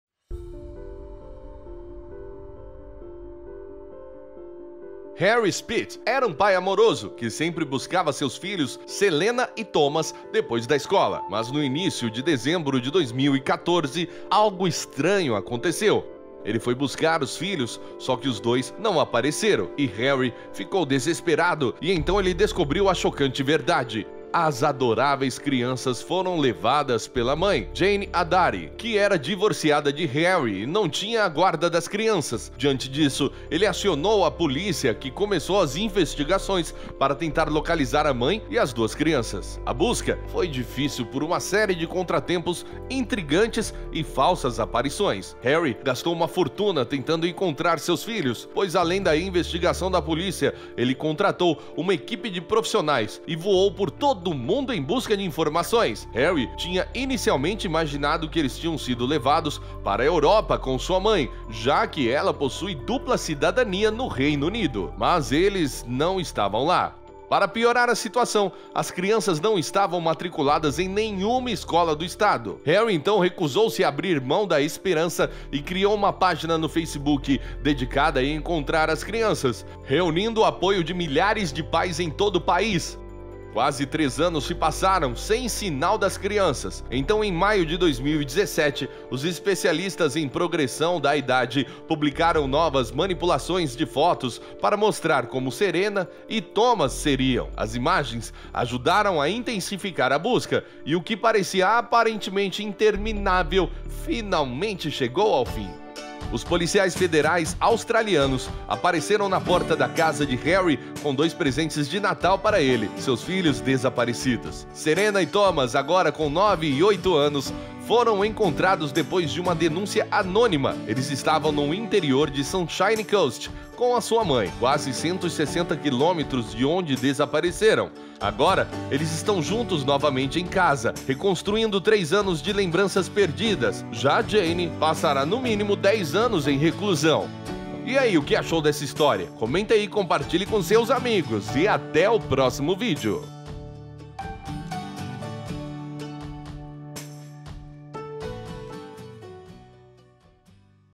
Irmãos SOMEM SEM DEIXAR VESTÍGIOS e 3 anos depois a POLÍCIA bate na porta do PAI e...
Nesse dias dos pais contaremos as história de Harry Speath era um pai amoroso que sempre buscava seus filhos Selena e Thomas depois da escola, mas no início de dezembro de 2014, algo estranho aconteceu.
Ele foi buscar os filhos só que os dois não apareceram e Harry ficou desesperado e então ele descobriu a chocante verdade, confira.

Aqui no Mata Curiosidade você encontra novidades, curiosidades, histórias de superação, histórias emocionantes e notícias bem interessantes!